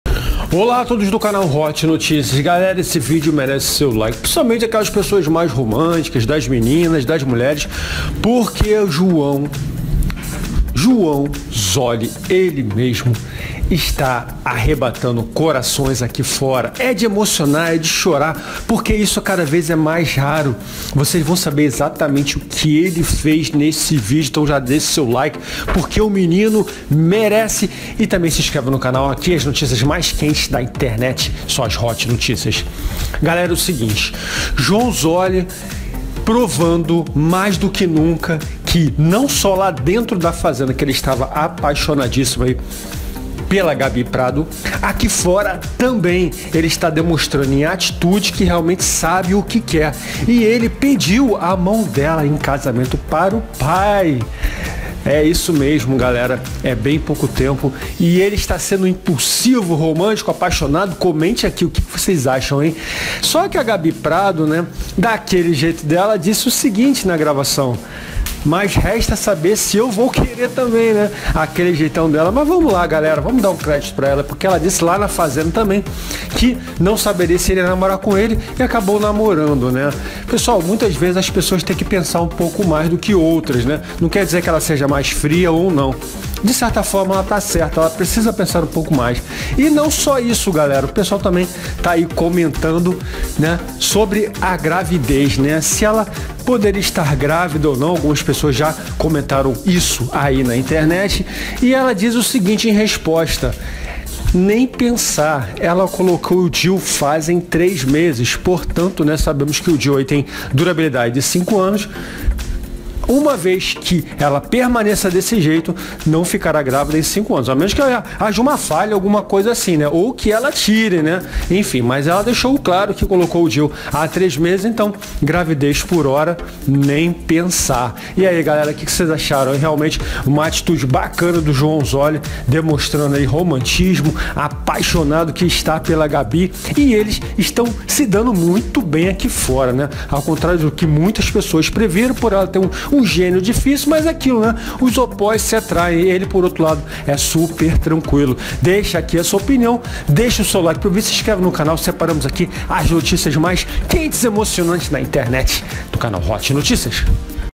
É DE CHORAR, SAIBA O Q ACONTECEU EM PEDIDO DE CASAMENTO DE JOÃO ZOLI P GABI APÓS FAZENDA 10 2018
SAIBA O Q ACONTECEU EM PEDIDO DE CASAMENTO DE JOÃO ZOLI P GABI  APÓS FAZENDA 10 2018